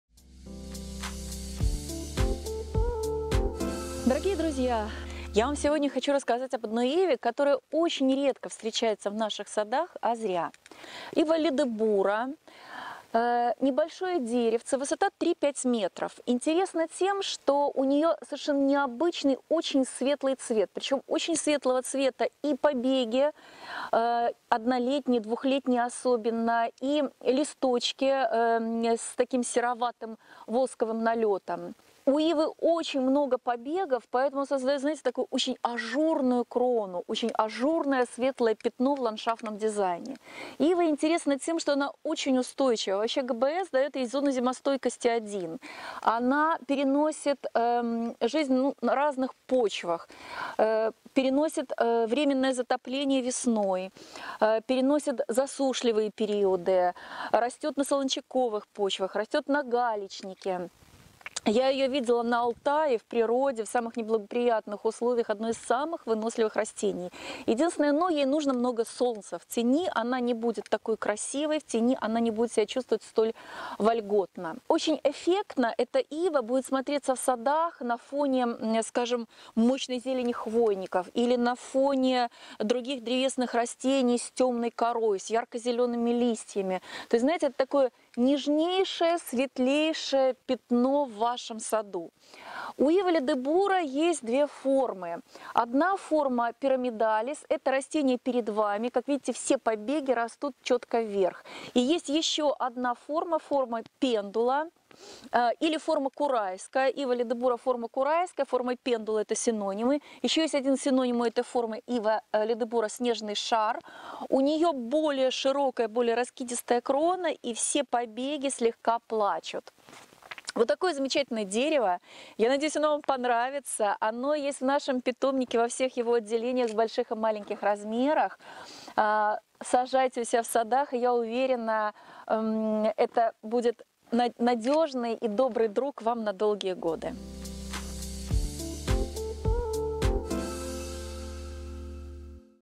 Ива Ледебура в "Питомнике Савватеевых"
Генеральный директор питомника - Ирина Савватеева, - с рассказом об Иве Ледебура (Пендула "Снежный шар" и "Пирамидалис")